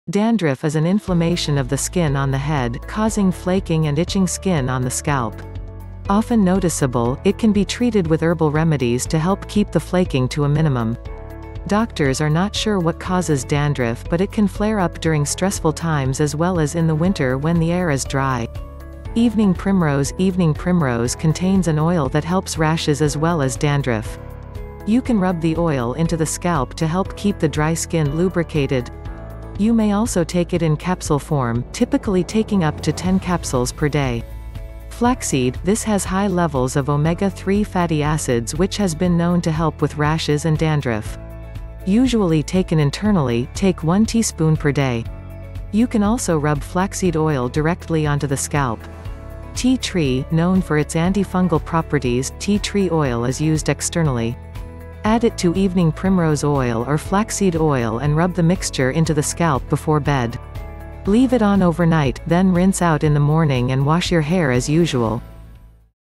Dandruff
Dandruff is an inflammation of the skin on the head, causing flaking and itching skin on the scalp. Often noticeable, it can be treated with herbal remedies to help keep the flaking to a minimum. Doctors are not sure what causes dandruff but it can flair up during stressful times as well as in the winter when the air is dry.
Evening Primrose - Evening primrose contains an oil that helps rashes as well as dandruff. You can rub the oil into the scalp to help keep the dry skin lubricated. You may also take it in capsule form, typically taking up to
10 capsules per day.
Flaxseed - This has high levels of omega-3 fatty acids which has been known to help with rashes and dandruff. Usually taken internally, take 1 teaspoon per day. You can also rub flaxseed oil directly onto the scalp.
Tea Tree - Known for its anti-fungal properties, tea tree oil is used externally. Add it to evening primrose oil or flaxseed oil and rub the mixture into the scalp before bed. Leave it on overnight, then rinse out in the morning and wash your hair as usual.